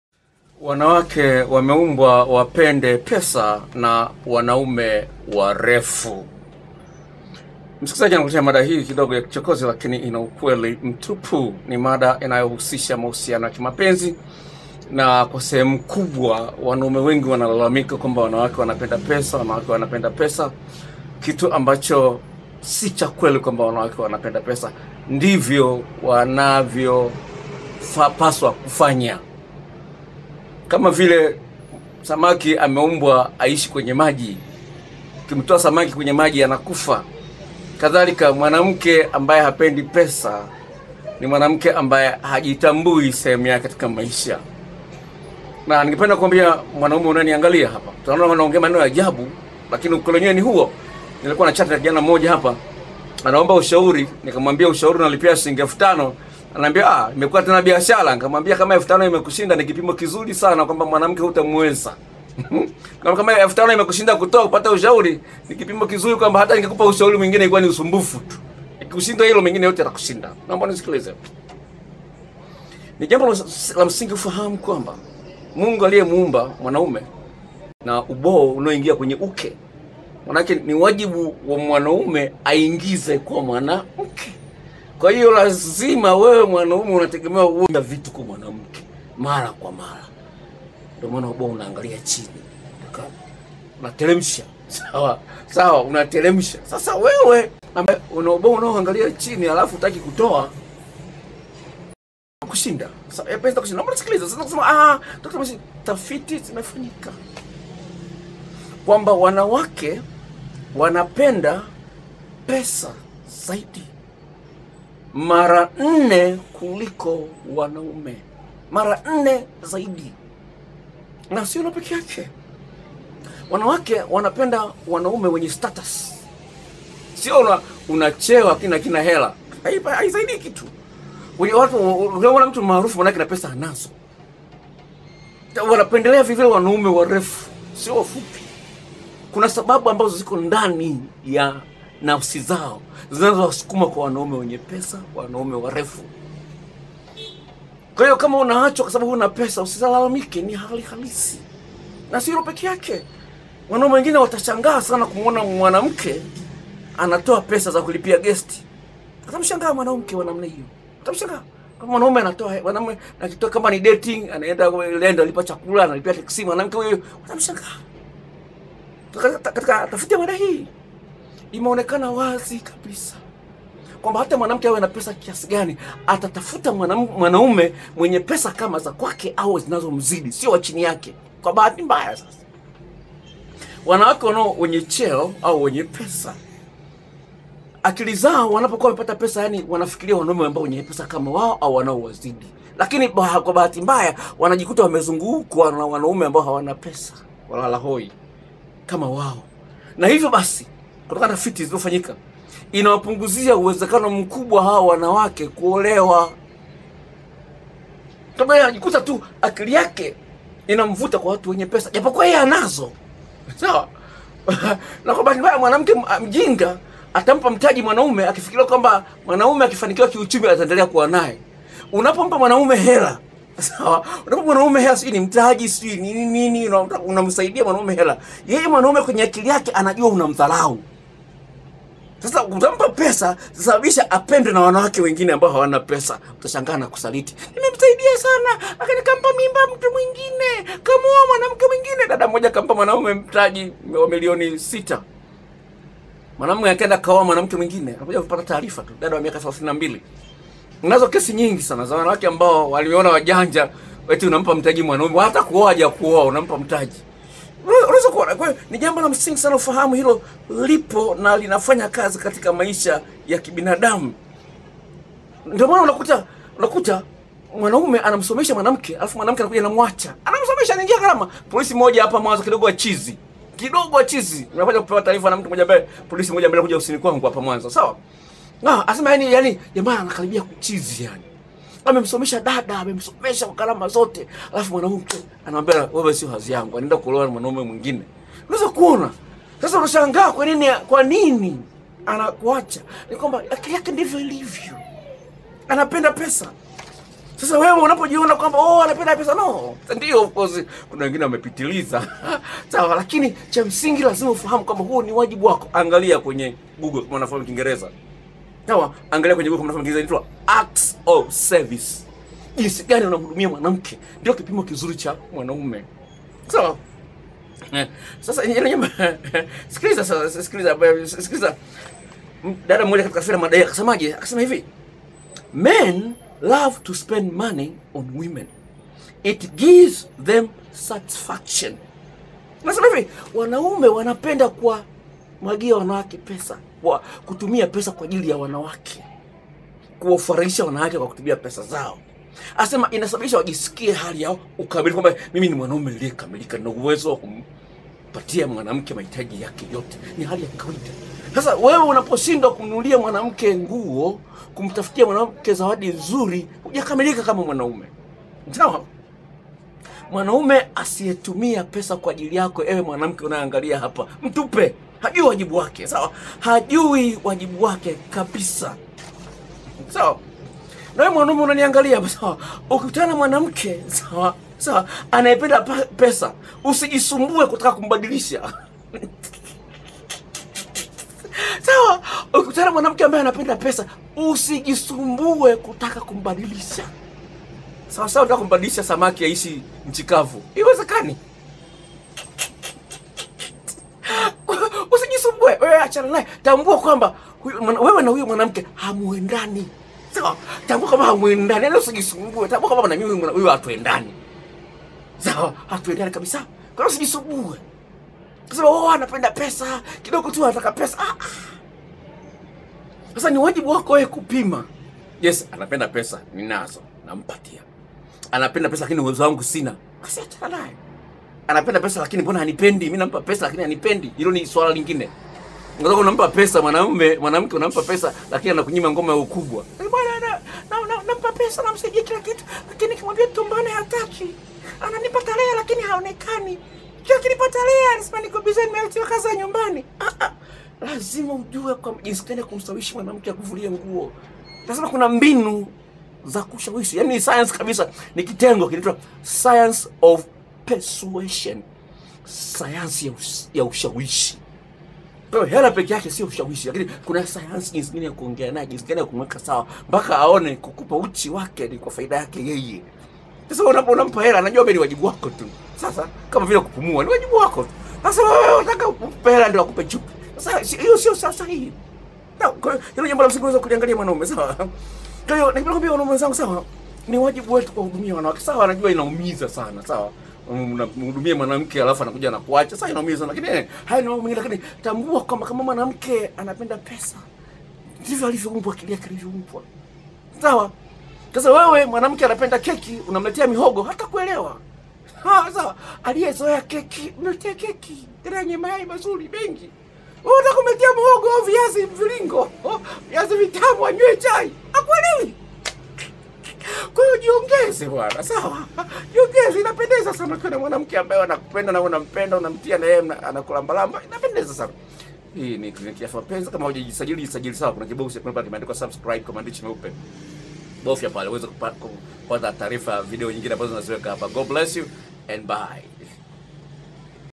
Wanawake wanapenda pesa na wanaume warefu tafiti zimethibitisha
Ukijaribu kumbadilisha mwanamke abadilishe mtazamo wake juu ya pesa utajiumiza mwenyewe